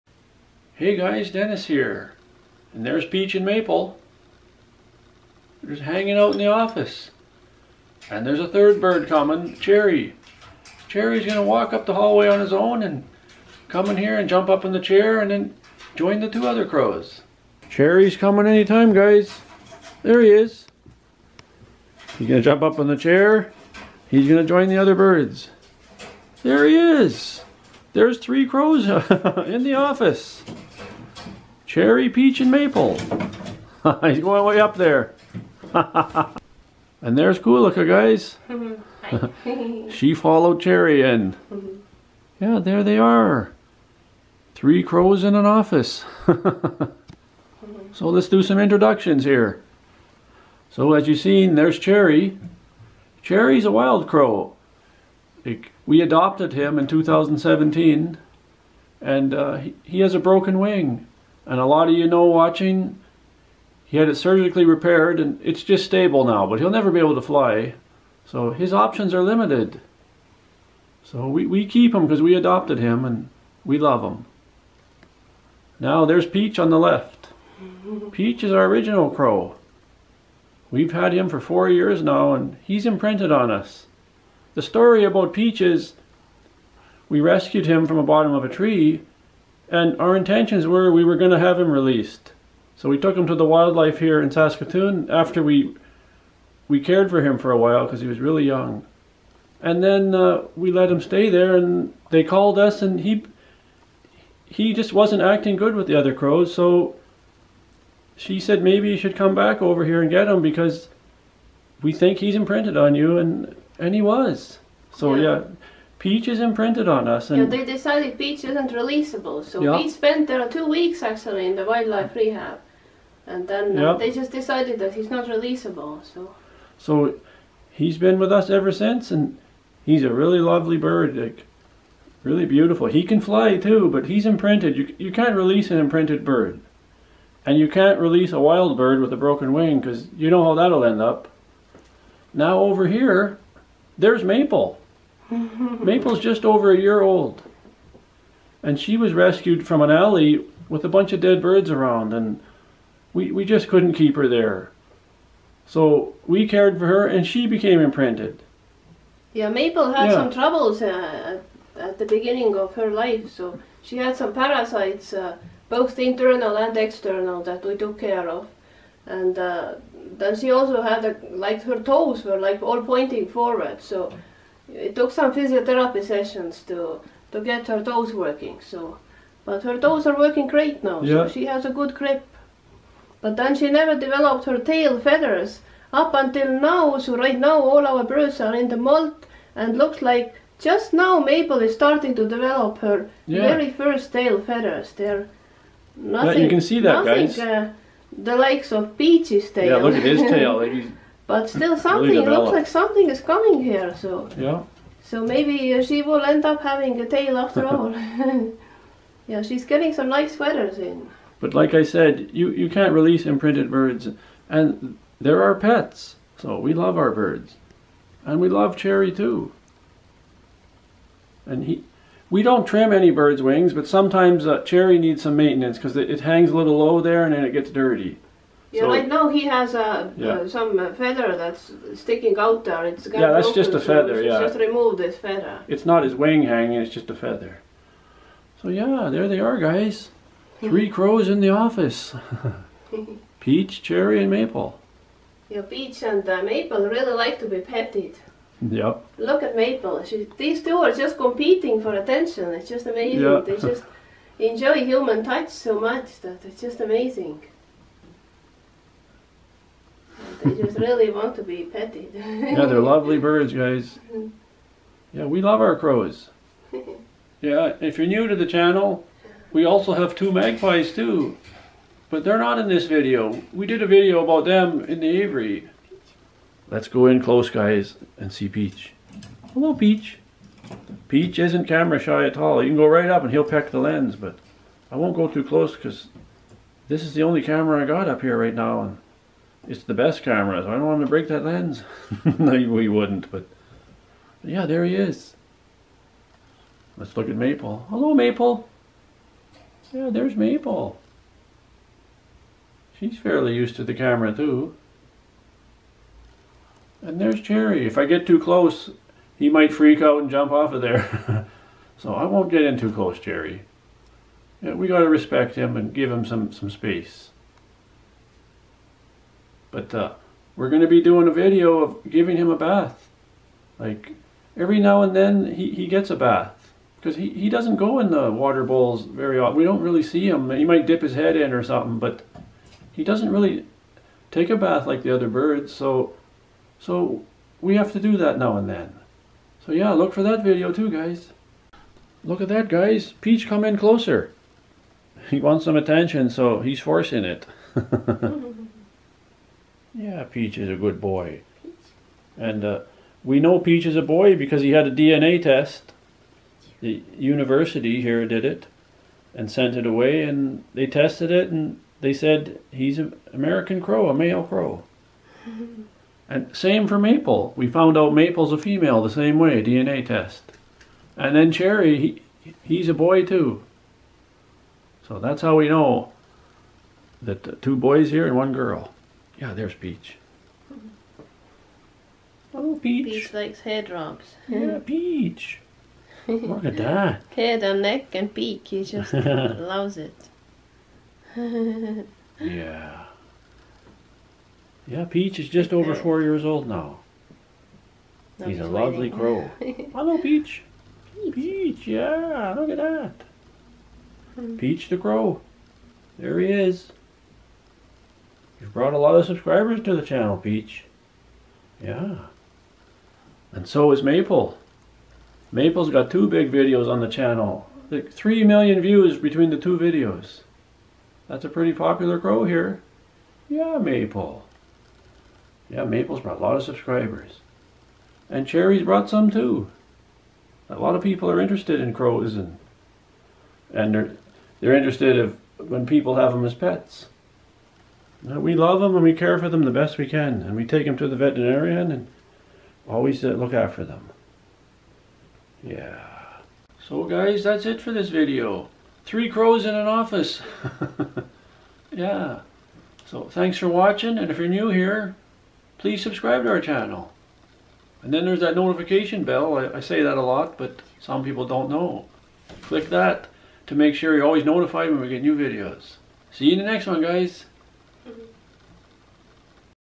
Three crows in an Office
This is a video of our 3 pet crows in an office. Peach, Cherry and Maple like hanging out in the office. Two of the crows are imprinted on us and the other crow was adopted from the wildlife rehab and cannot fly.